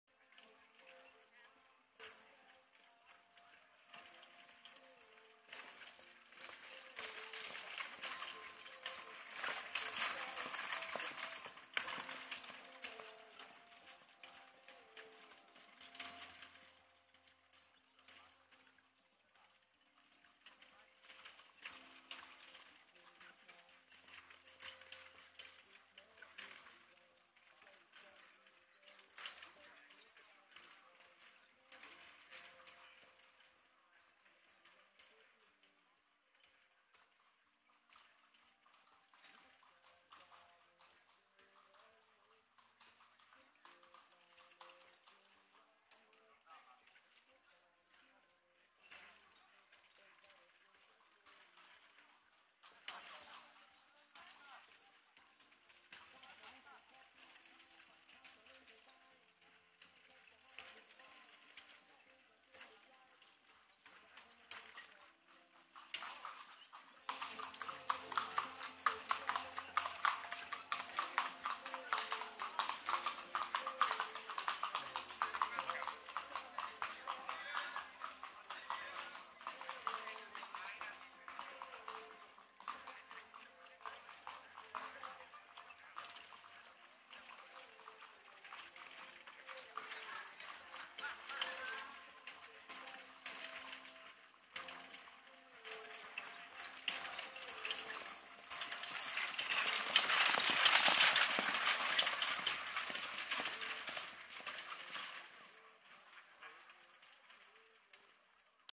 Fahrturnier Emlichheim 21.8.2021
Ein kurzes Video einer Kegel-Fahr-Prüfung vom Fahrturnier in Emlichheim
Samstag